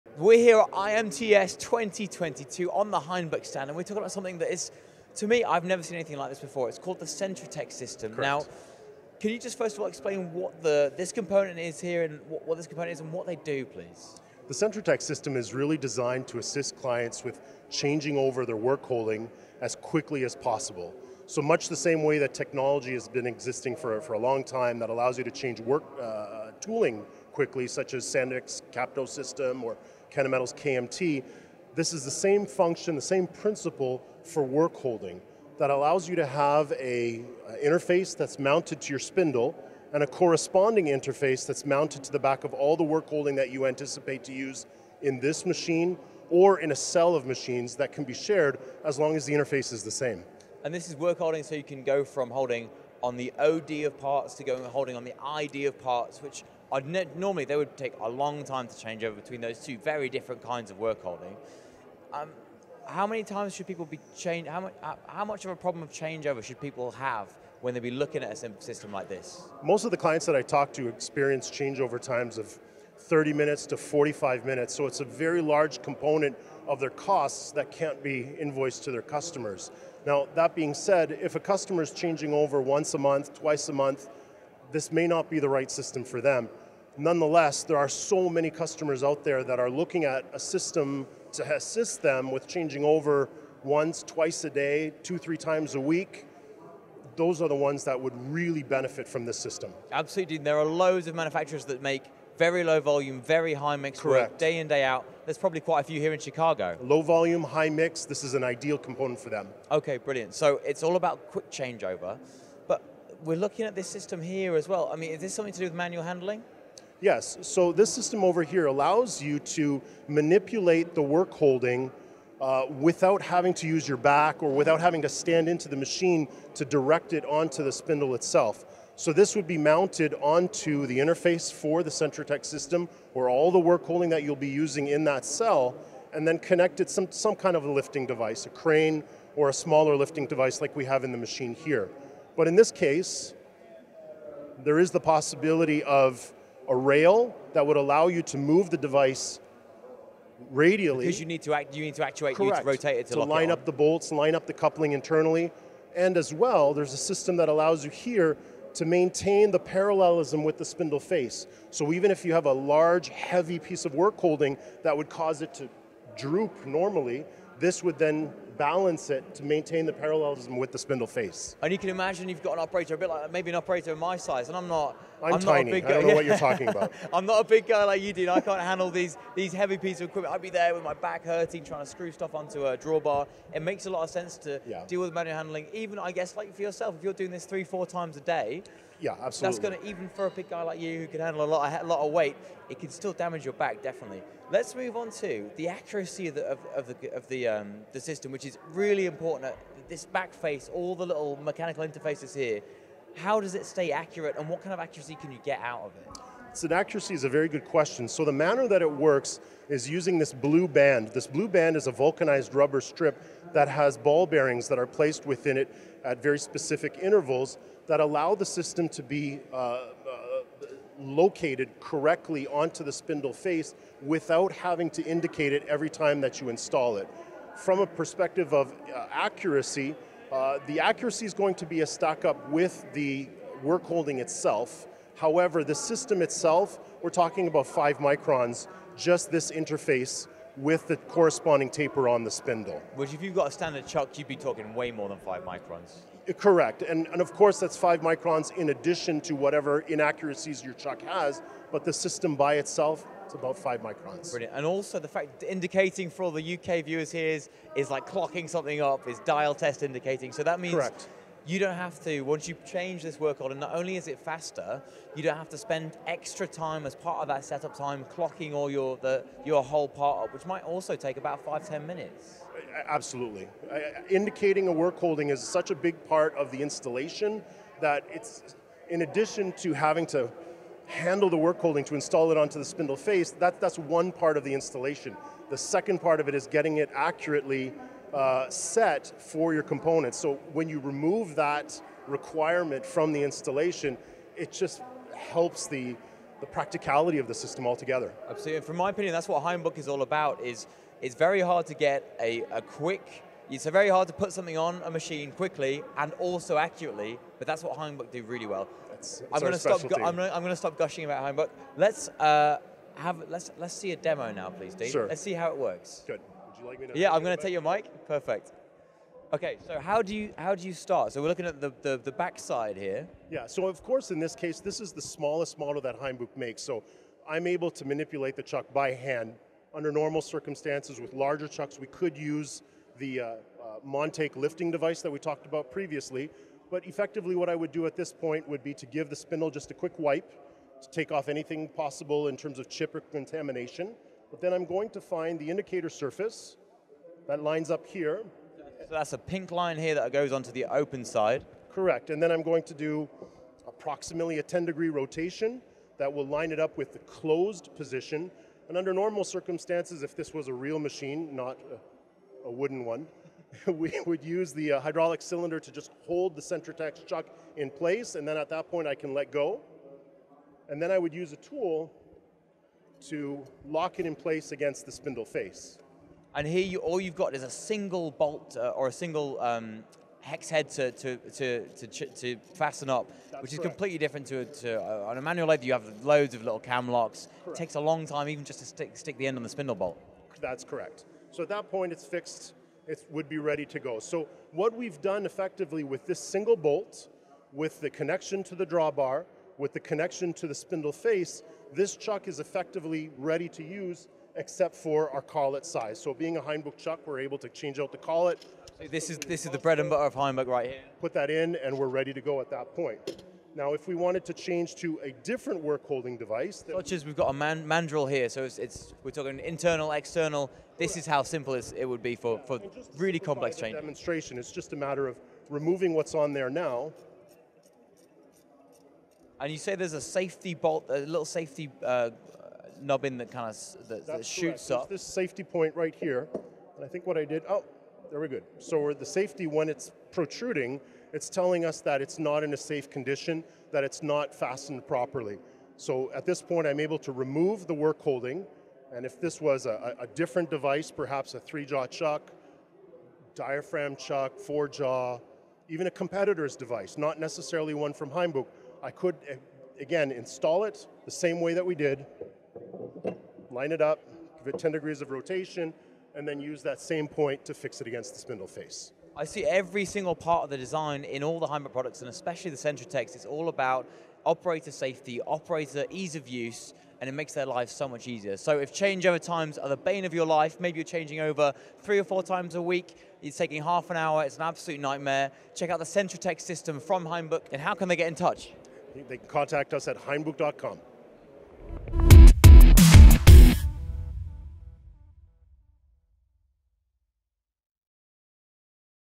Make changeover times quicker and easier with HAINBUCH’s CentroteX
Rowan Easter-Robinson from MTDCNC visits the HAINBUCH stand at IMTS 2022 for a quick-change demonstration with Dean Winkel. HAINBUCH’s CentroteX system is designed to assist customers with changing over as quickly as possible. It includes an interface mounted to the spindle and a corresponding interface mounted to the back of all the workholding to be used in the machine or in a cell of machines. Customers that are changing over once twice a day or two to three times a week would benefit immensely from this system, given it’s an ideal component for low-volume, high-mix production. Offering an incredible accuracy of 5 microns, the system also delivers the same safety features and ease of use as all other HAINBUCH products.